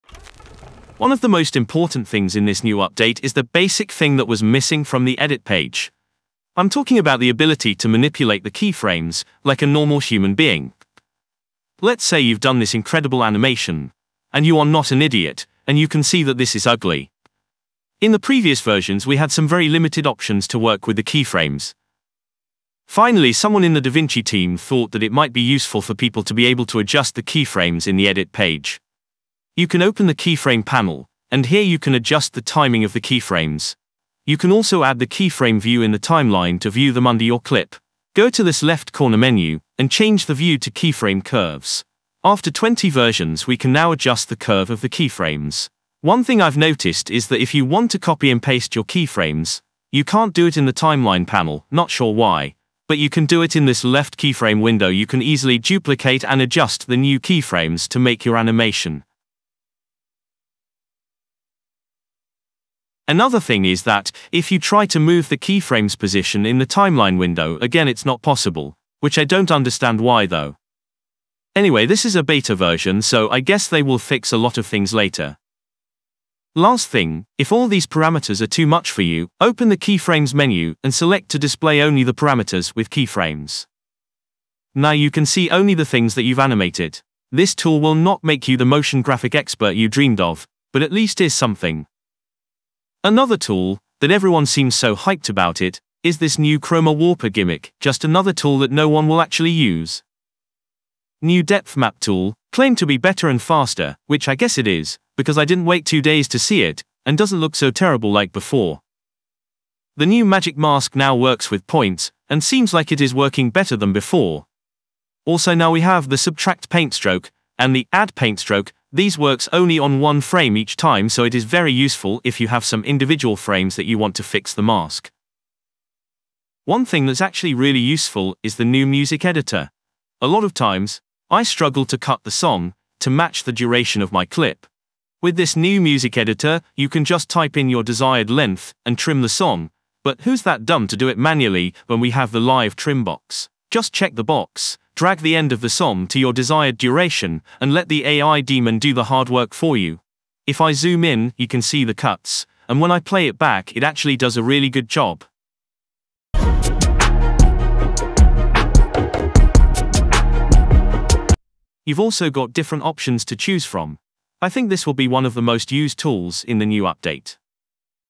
Is There Anything Good In Davinci Resolve 20 ?
Just a quick view in the new tools of davinci resolve 20

You can send them here :

BTC : 1Ecg6F8UnFHnVxsKhbkecqtge11vBhqS5
ETH: 0xB643C1C8B6C75bcB3426F40f8514CB89Fc294abC